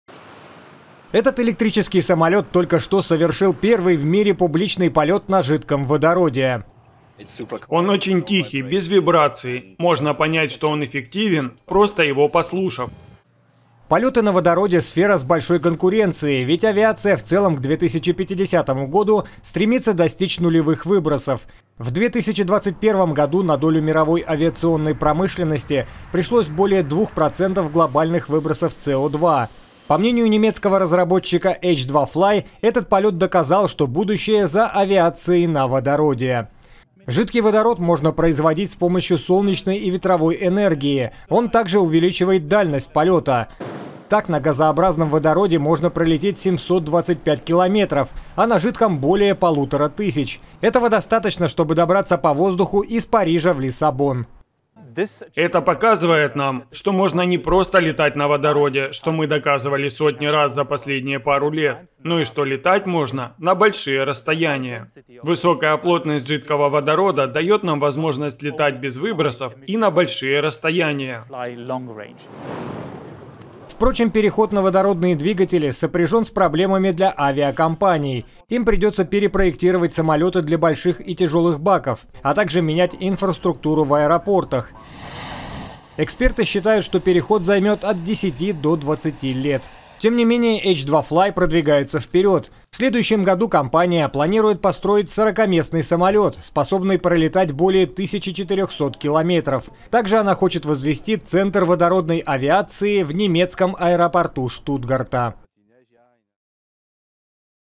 Первый публичный полёт на жидком водороде: H2FLY идёт к «зелёной» авиации
Немецкий авиаразработчик H2FLY успешно выполнил первый публичный полёт электрического самолета, работающего на жидком водороде. Об этом подробнее.